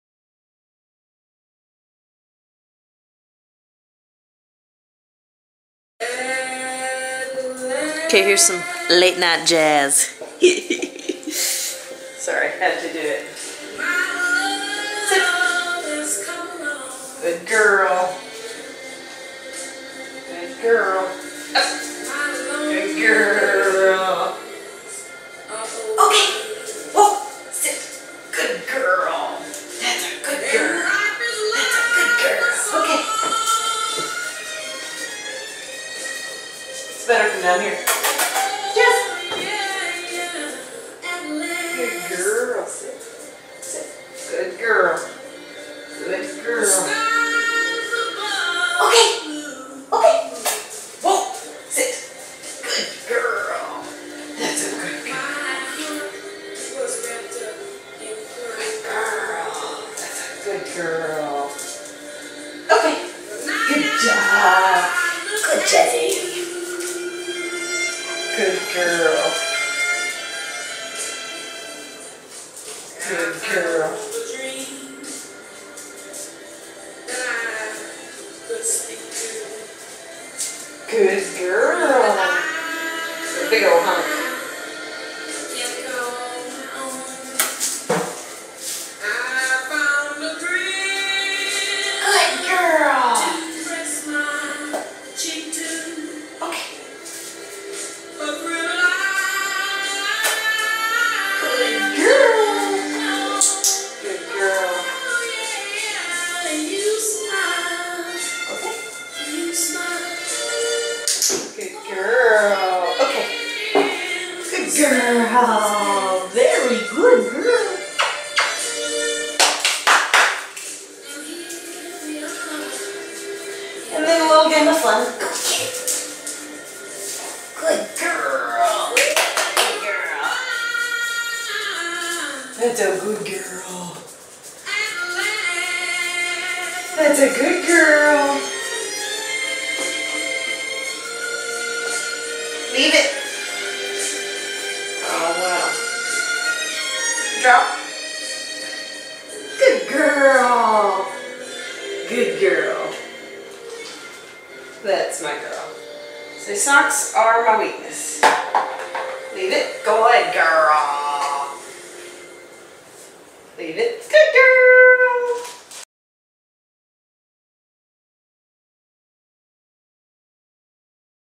Late Night Jazz, Vol I
Random training with Jazzie girl & some Etta James for easy listening . . .